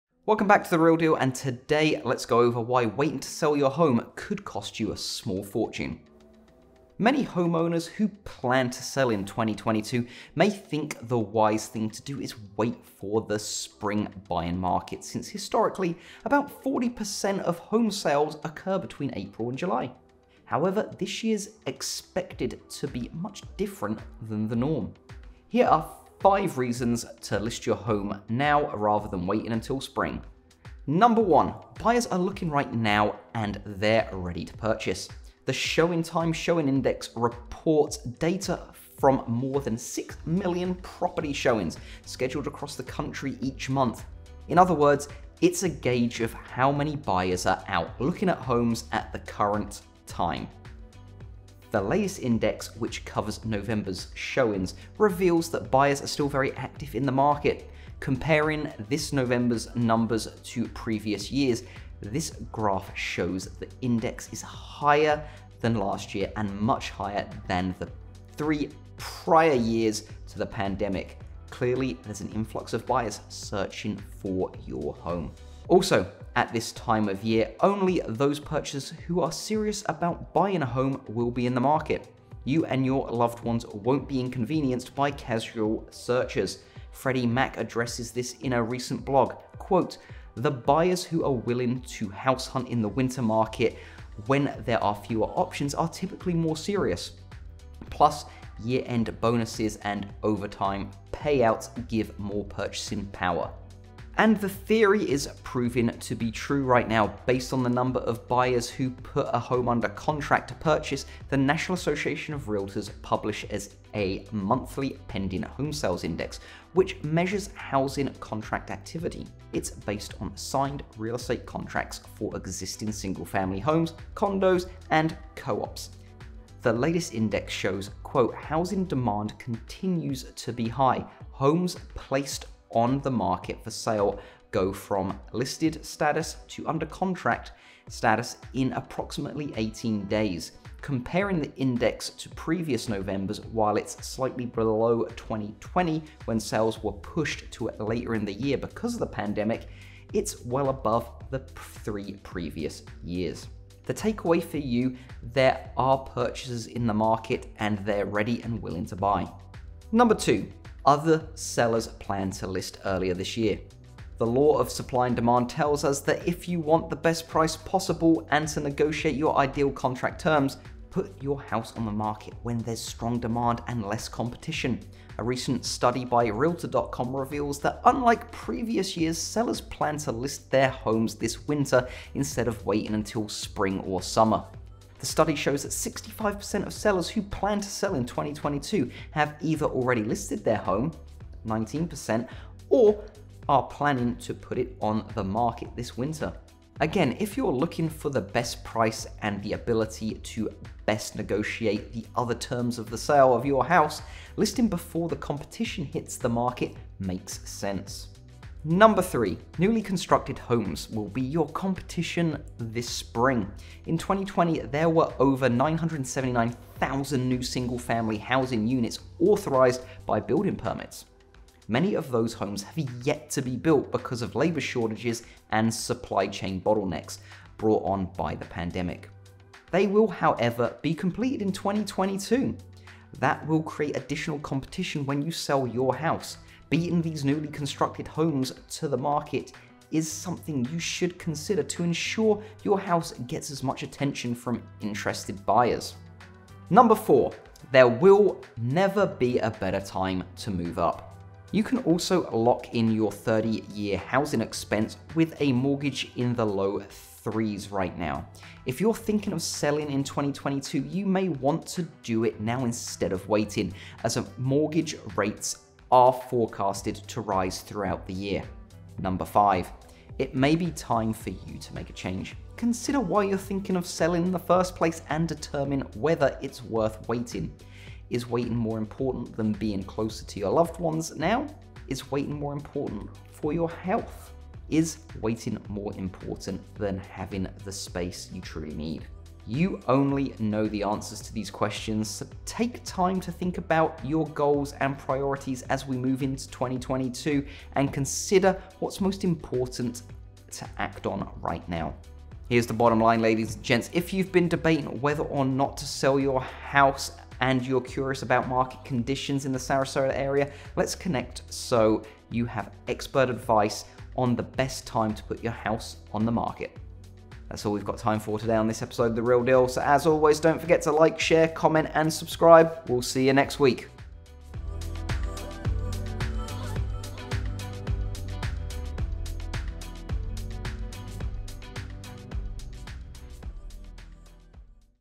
Why Waiting To Sell Your House Could Cost You a Small Fortune
Here are five reasons to list your house now rather than waiting until the spring.
1. Buyers Are Looking Right Now, and They’re Ready To Purchase
2. Other Sellers Plan To List Earlier This Year
3. Newly Constructed Homes Will Be Your Competition in the Spring
4. There Will Never Be a Better Time To Move-Up
5. It May Be Time for You To Make a Change

If you’ve been debating whether or not to sell your house and are curious about market conditions in your area, let’s connect so you have expert advice on the best time to put your house on the market.

0:00 Intro
0:31 Buyers Are Looking Right Now, and They’re Ready To Purchase
2:42 Other Sellers Plan To List Earlier This Year
3:29 Newly Constructed Homes Will Be Your Competition in the Spring
4:08 There Will Never Be a Better Time To Move-Up
4:31 It May Be Time for You To Make a Change
5:03 The Bottom Line